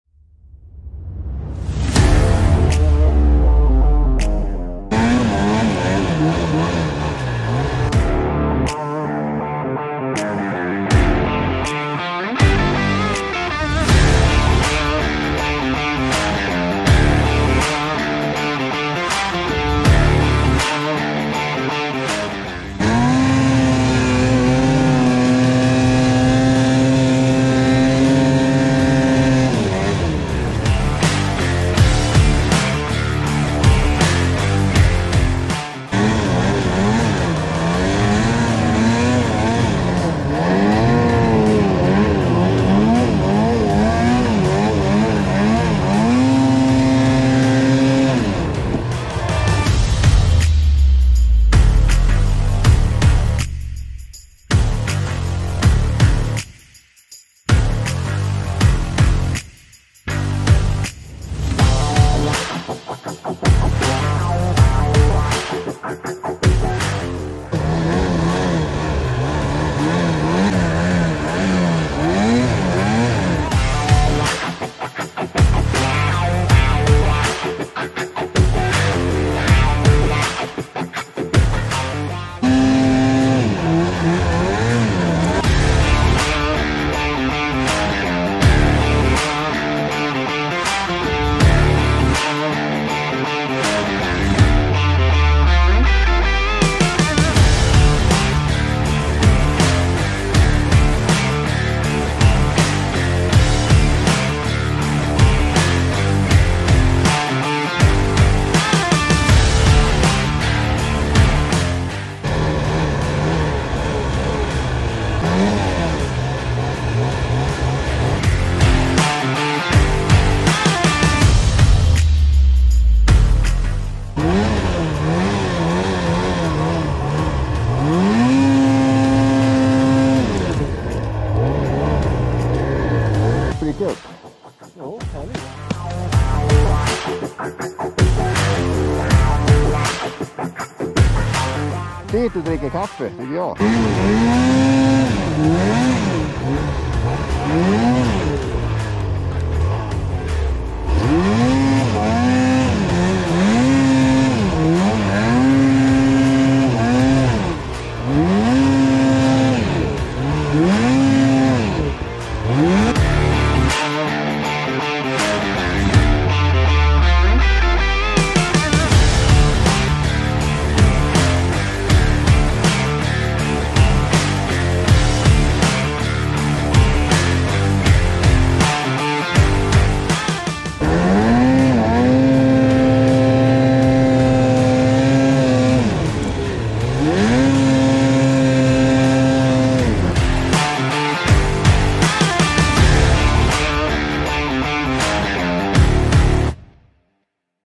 Kittelfjäll (10/1 2021)
Premiär! Ganska bra med snö, dock ingen botten. Man fick se sig för var man sätter skidorna.

Ski-doo Summit XM 163
Lynx Boondocker 3900